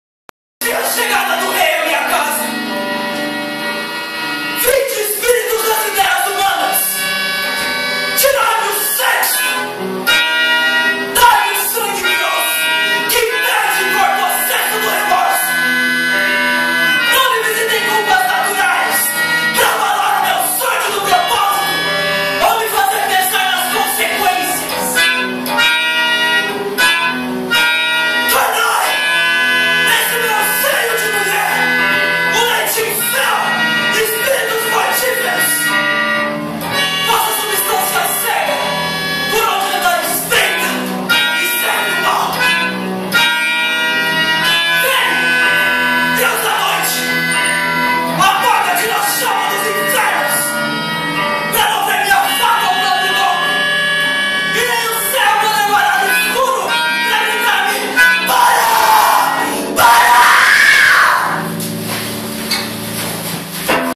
MACBETH - Escola RecriArte - 61ª Mostra de Artes Cênicas
A RecriArte - Escola de arte e comunicação e a Turma do Projeto de Montagem Macbeth 2012
apresentam: MACBETH